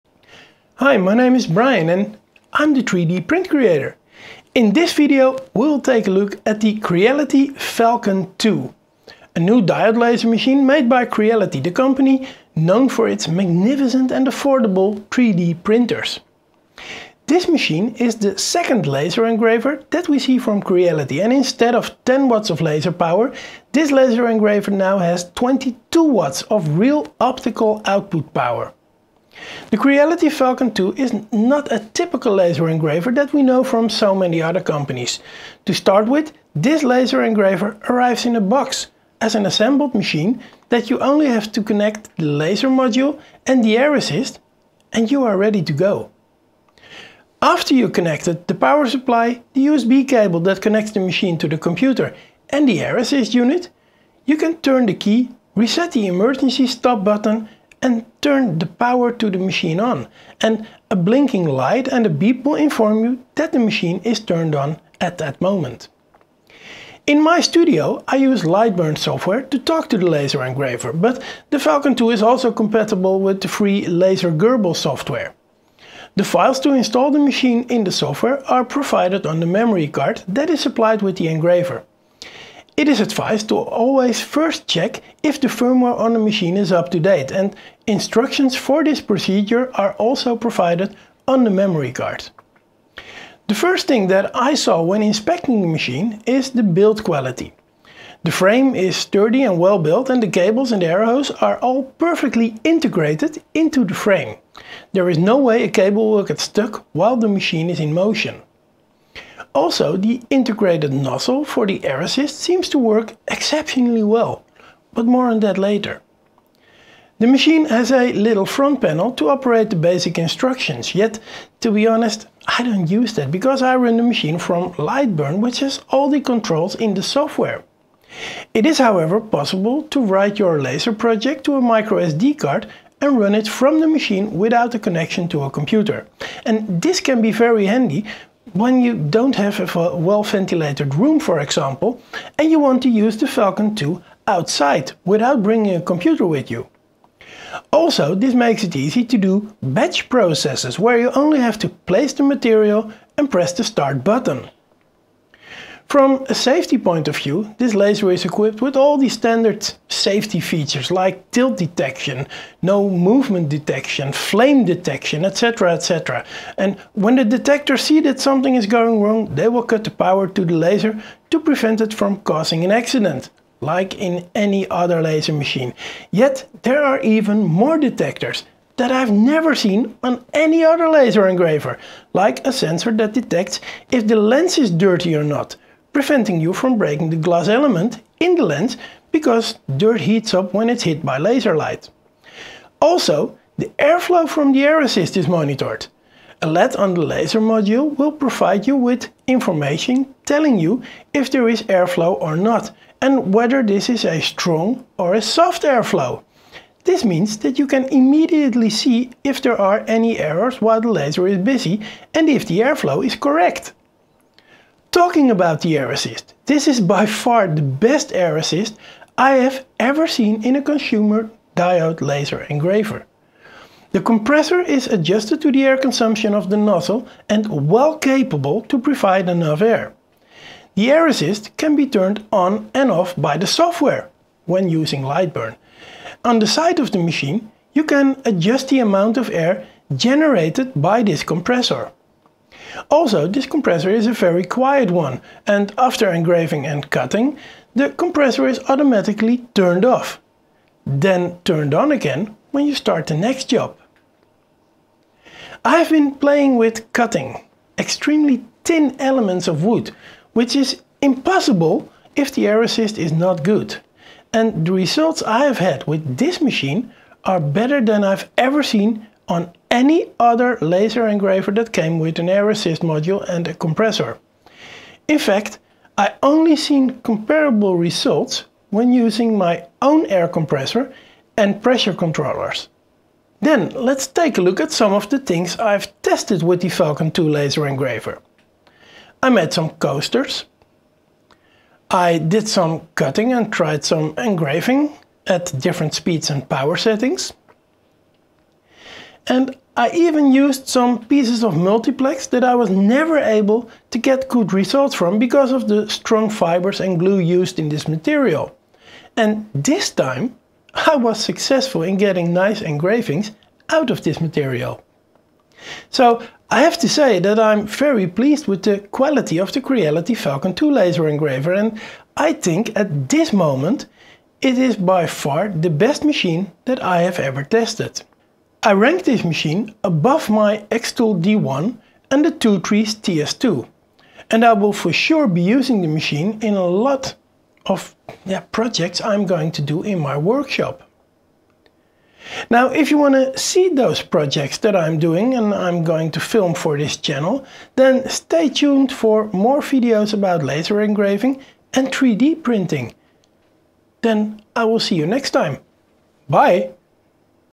Let's Get Engraving With The Creality Falcon 2 Laser Engraver!
𝗕𝘂𝘆 𝘁𝗵𝗲 𝗖𝗿𝗲𝗮𝗹𝗶𝘁𝘆 𝗙𝗮𝗹𝗰𝗼𝗻𝟮 𝗹𝗮𝘀𝗲𝗿 𝗵𝗲𝗿𝗲:
Link1：
In this video, we look at the Creality Falcon 2 Laser Engraver, a 22 Watt laser machine that stands head and shoulders above all other laser engraving machines.
The Creality Falon2 has enough power to cut through a thick piece of wood, but it is also capable of making detailed engravings. The laser comes with a very good quality Air Assist, which means that the Falcon2 is also able to cut very thin objects. The safety measures in the Creality Falcon2 are exceptional because this laser has many more sensors to detect problems than any other machine I've ever tested. For example, the Falcon 2 has a sensor to see if the lens is dirty, and another sensor to see how good the airflow to the air-assist nozzle is.
The Creality Falcon2 is certainly not a cheap machine, but it is the first diode laser engraving machine that can actually be used in a production environment, making it much better than other lasers that you should mainly see as hobby machines.